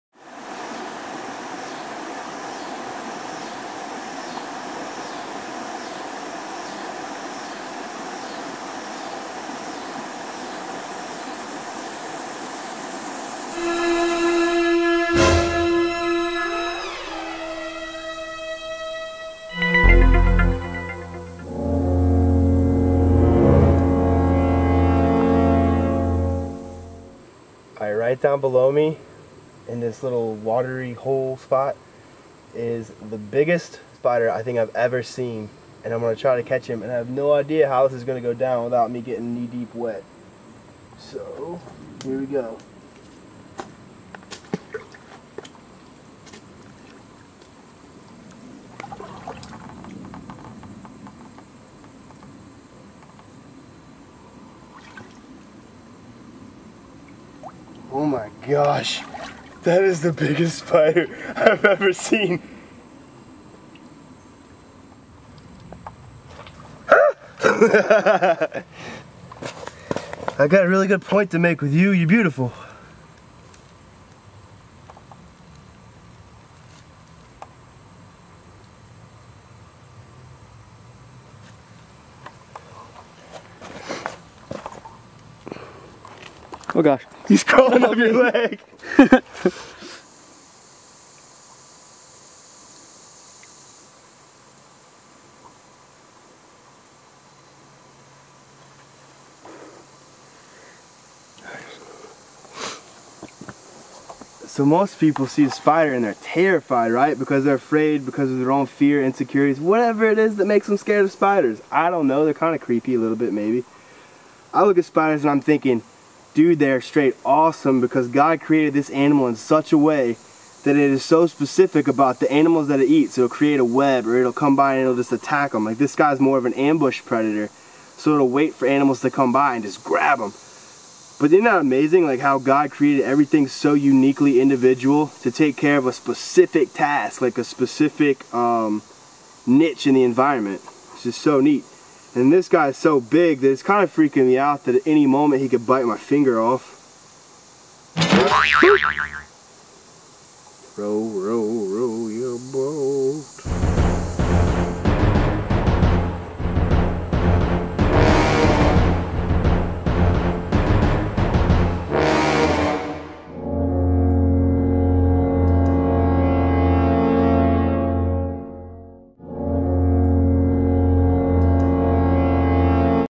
FishingSpider
This is  probably the biggest spider i have ever seen in North Carolina!  God created it for a specific purpose and it is so cool!

Colossians 1:16 (New International Version)

For by him all things were created: things in heaven and on earth, visible and invisible, whether thrones or powers or rulers or authorities; all things were created by him and for him.